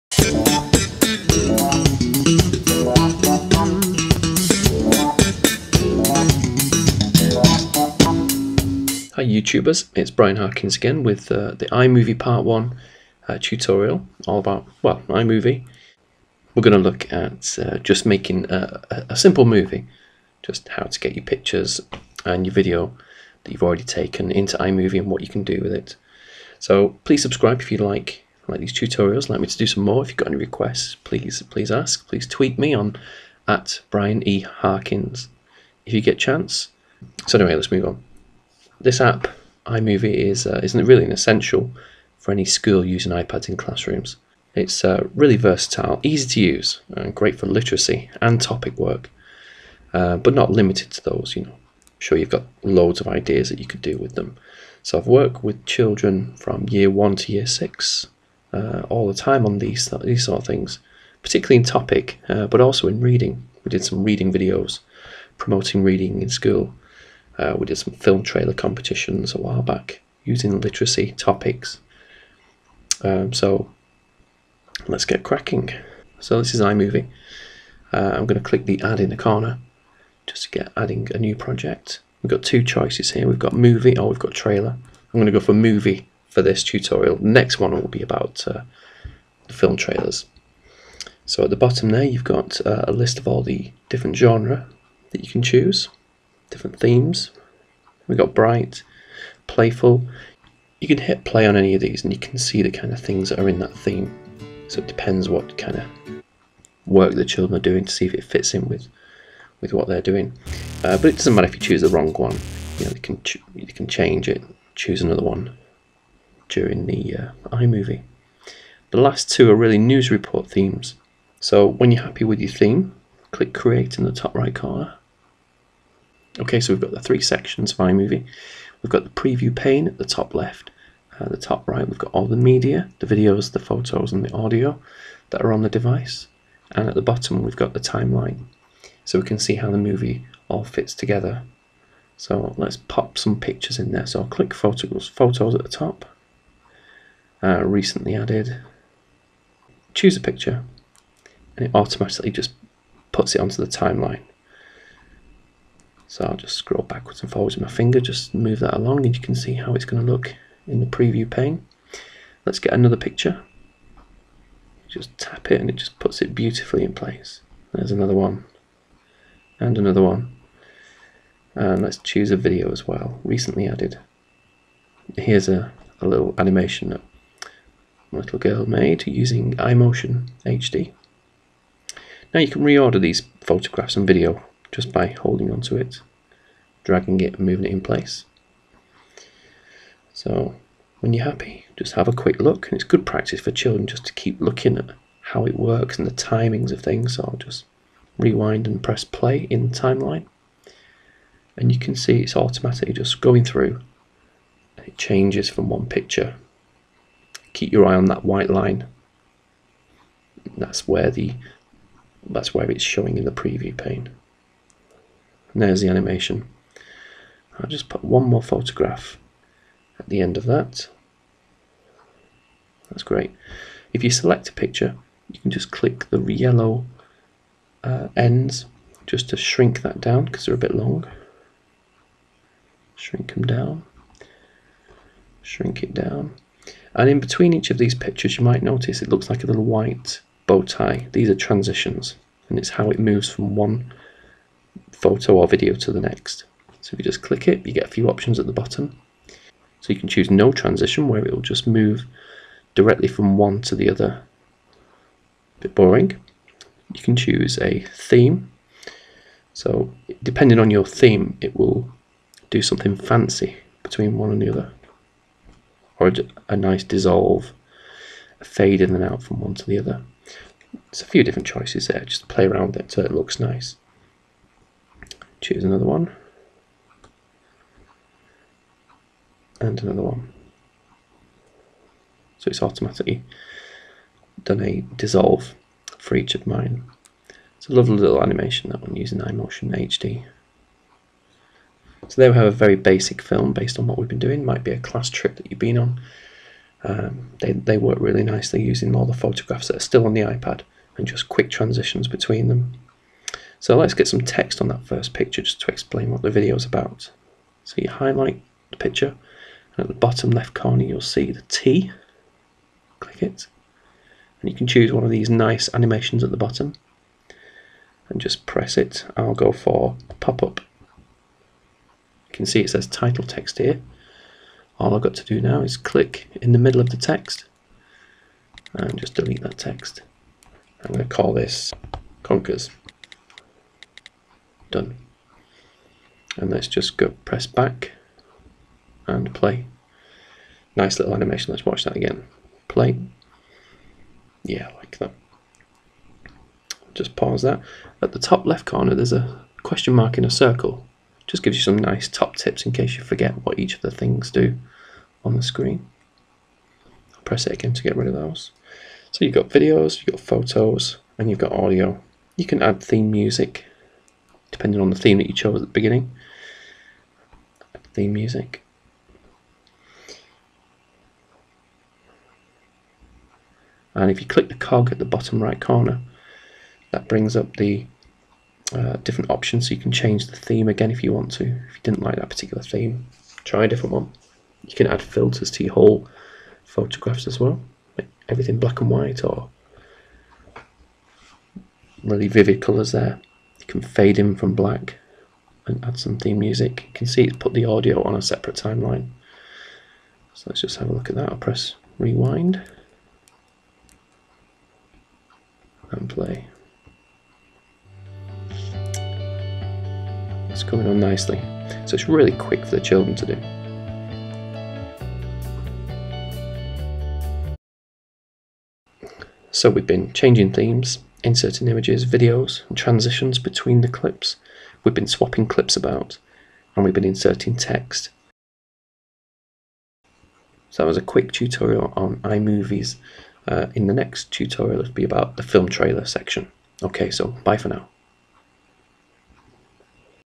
iPad Classroom Tutorial - iMovie Part 1 Making Movies
This is part one of my iMovie in the Classroom Tutorials. I will demonstrate some of the main features of the Movie section and how to use photos and video, with transitions to make a simple movie. Add text? Why not and here's how...